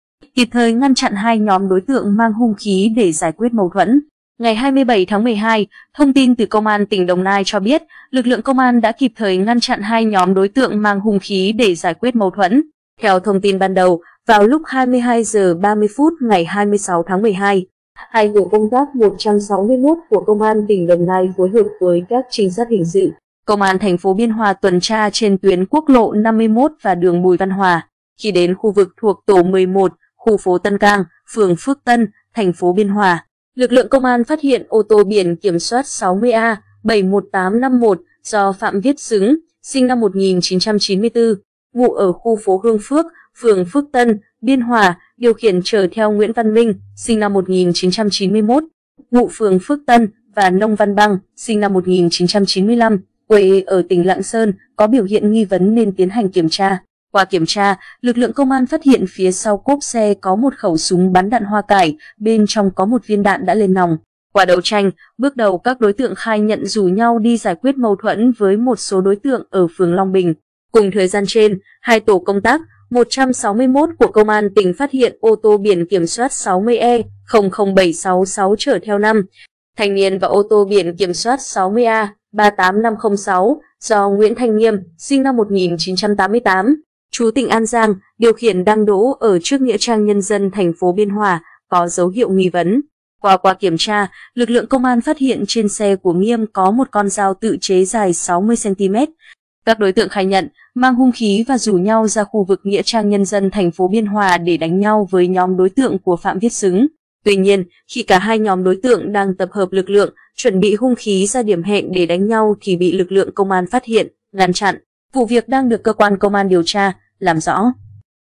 Kịp thời ngăn chặn 2 nhóm đối tượng mang hung khí để giải quyết mâu thuẫn
Ngày 27/12, thông tin từ Công an tỉnh Đồng Nai cho biết, lực lượng Công an đã kịp thời ngăn chặn 2 nhóm đối tượng mang hung khí để giải quyết mâu thuẫn.Cùng thời gian trên, hai tổ công tác 161 của Công an tỉnh phát hiện ô tô biển kiểm soát 60E- 00766 chở theo 5 thanh niên và ô tô biển kiểm soát 60A-38506 do Nguyễn Thanh Nghiêm (sinh năm 1988, trú tỉnh An Giang) điều khiển đang đỗ ở trước nghĩa trang nhân dân thành phố Biên Hòa, có dấu hiệu nghi vấn.
Vụ việc đang được cơ quan Công an điều tra, làm rõ.
Dao tự chế của đối tượng Nguyễn Thanh Nghiêm. Ảnh: TTXVN phát
Qua qua kiểm tra, lực lượng Công an phát hiện trên xe của Nghiêm có một con dao tự chế dài 60cm. Các đối tượng khai nhận, mang hung khí và rủ nhau ra khu vực Nghĩa trang nhân dân thành phố Biên Hòa để đánh nhau với nhóm đối tượng của Phạm Viết Xứng. Tuy nhiên, khi cả 2 nhóm đối tượng đang tập hợp lực lượng, chuẩn bị hung khí ra điểm hẹn để đánh nhau thì bị lực lượng Công an phát hiện, ngăn chặn.
Nhóm đối tượng đi trên xe ô tô mang biển số 60E- 00766. Ảnh: TTXVN phát
Đối tượng Phạm Viết Xứng. Ảnh: TTXVN phát
Theo thông tin ban đầu, vào lúc 22 giờ 30 phút ngày 26/12, hai tổ công tác 161 của Công an tỉnh Đồng Nai phối hợp với các trinh sát hình sự, Công an thành phố Biên Hòa tuần tra trên tuyến Quốc lộ 51 và đường Bùi Văn Hòa. Khi đến khu vực thuộc tổ 11, khu phố Tân Cang, phường Phước Tân, thành phố Biên Hòa, lực lượng Công an phát hiện ô tô biển kiểm soát 60A-718.51 do Phạm Viết Xứng (sinh năm 1994, ngụ ở khu phố Hương Phước, phường Phước Tân, Biên Hòa) điều khiển chở theo Nguyễn Văn Minh (sinh năm 1991, ngụ phường Phước Tân) và Nông Văn Băng (sinh năm 1995, quê ở tỉnh Lạng Sơn) có biểu hiện nghi vấn nên tiến hành kiểm tra. Qua kiểm tra, lực lượng Công an phát hiện phía sau cốp xe có 1 khẩu súng bắn đạn hoa cải, bên trong có 1 viên đạn đã lên nòng.
Qua đấu tranh, bước đầu các đối tượng khai nhận rủ nhau đi giải quyết mâu thuẫn với một số đối tượng ở phường Long Bình.
Khẩu súng của đối tượng Phạm Viết Xứng. Ảnh: TTXVN phát